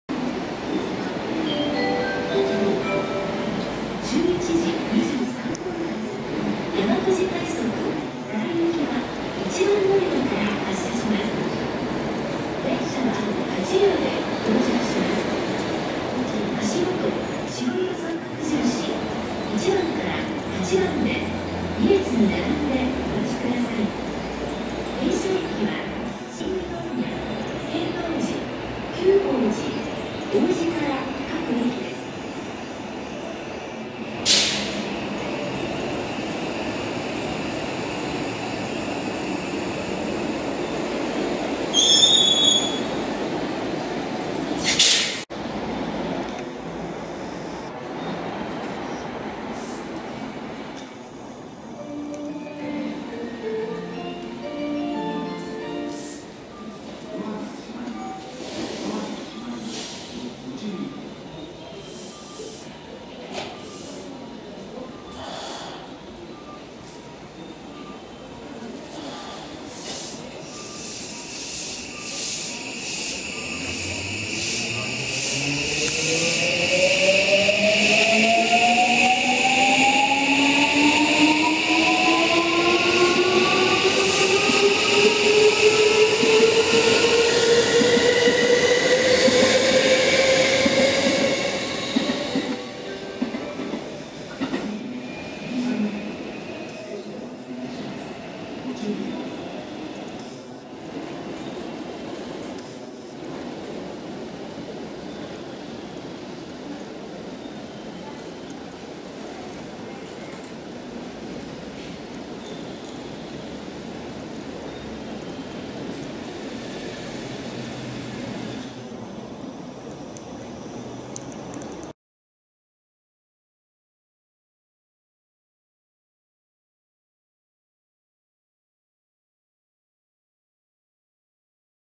大阪環状線 大正駅に外回りの323系が到着～発車まで
JR大阪環状線の大正駅に外回りの323系が到着して来るところから発車していく所までを撮影した動画です。
323系はオレンジ色を使っている面積は少ないですが結構オレンジが主張している気がして悪くないと思います。